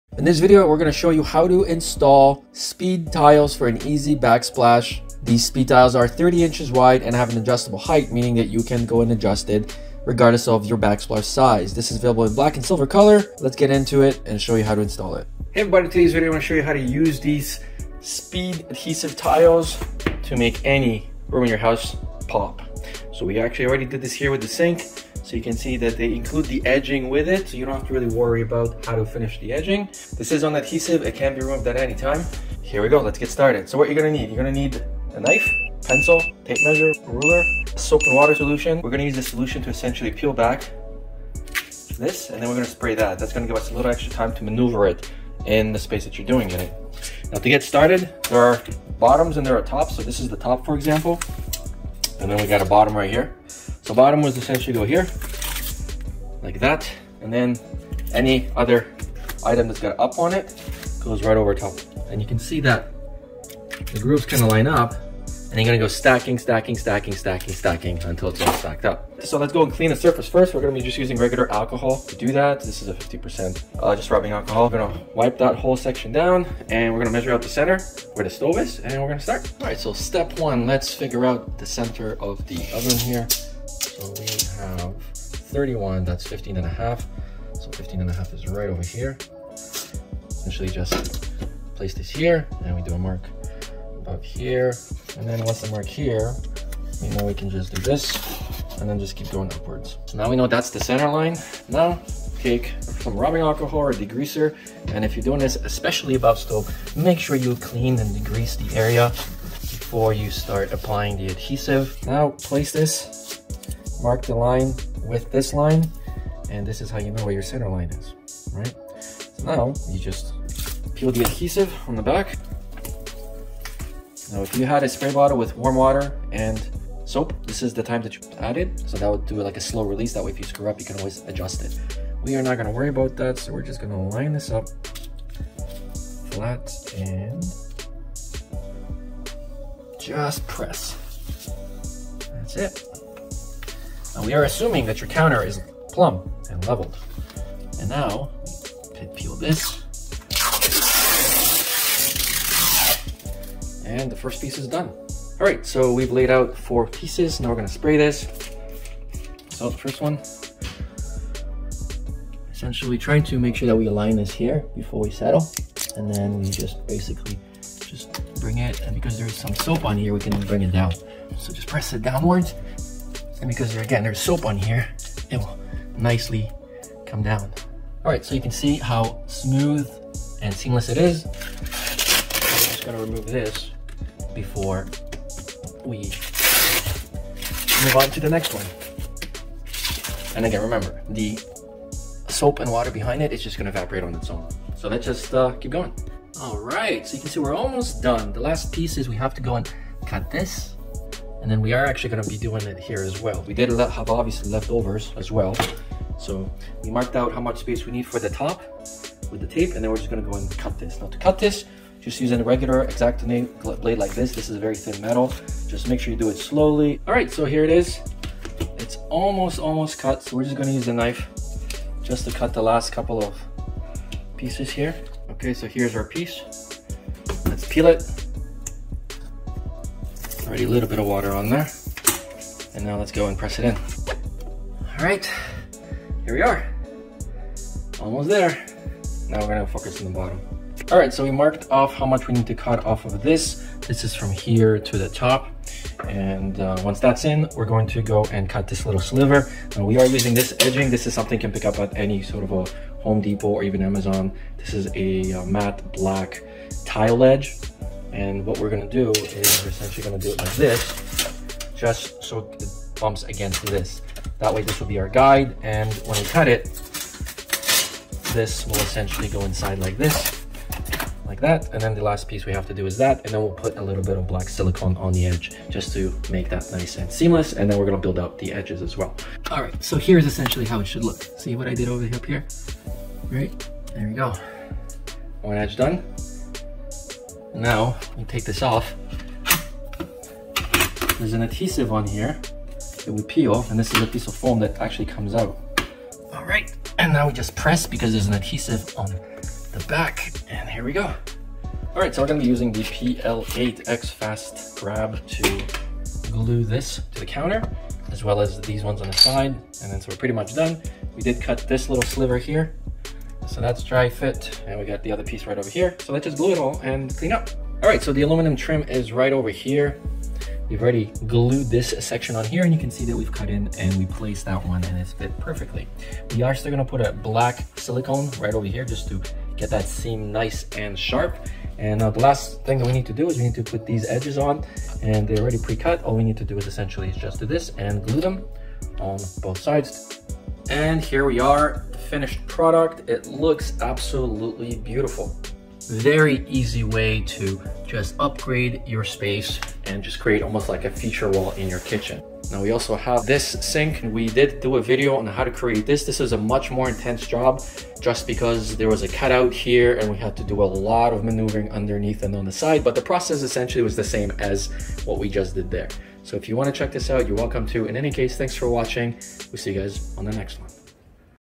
SpeedTiles Review & Installation - DIY Kitchen Backsplash with Peel and Stick Metal Tiles
- Today's video is a step by step guide on how to create a kitchen featured wall backsplash behind your stove in the kitchen and a speedtiles review and installation video. Black Hexagon Peel and Stick Backsplash, Self-Adhesive Aluminum Metal Surface Waterproof Stick on Wall Tile for Kitchen (This process can be used to Tile Countertops, Full Walls and entire kitchen backsplashes. The tiles aren't cheap but they are waterproof, Stain proof and will never rust as there is cork and thing layer of reflective metal. Cut it with a sharp contractor knife or use a wet saw. There is an adhesive on the back to stick to metal laminate, wood, vinyl or laminated wood and other smooth surfaces. You can also buy edging kits to cover the side imperfections for a clean look and you'll be extremely happy with the results and speed of how long it takes. DIY 30 Min Kitchen Backsplash - Kitchen Makeover with Peel and Stick Metal Tiles, DIY Speed Tiles